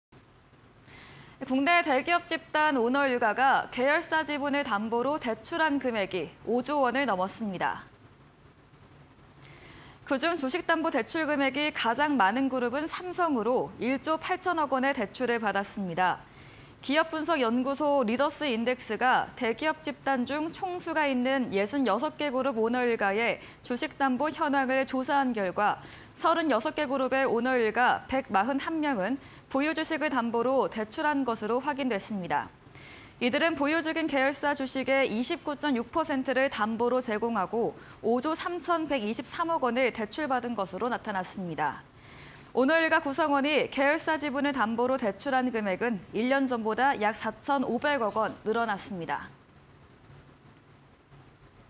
오너일가 주식담보대출 5조원 넘어… 1위는 삼성 (20220927)
국내 대기업 집단 오너 일가가 계열사 지분을 담보로 대출한 금액이 5조원을 넘었습니다.

그중 주식담보대출 금액이 가장 많은 그룹은 삼성으로 1조8000억원의 대출을 받았습니다.

기업분석연구소 리더스인덱스가 대기업 집단 중 총수가 있는 66개 그룹 오너일가의 주식담보 현황을 조사한 결과, 36개 그룹의 오너 일가 141명은 보유 주식을 담보로 대출한 것으로 확인됐습니다.

이들은 보유 중인 계열사 주식의 29.6%를 담보로 제공하고 5조3123억원을 대출받은 것으로 나타났습니다.

오너일가 구성원이 계열사 지분을 담보로 대출한 금액은 1년 전보다 약 4500억원 늘어났습니다.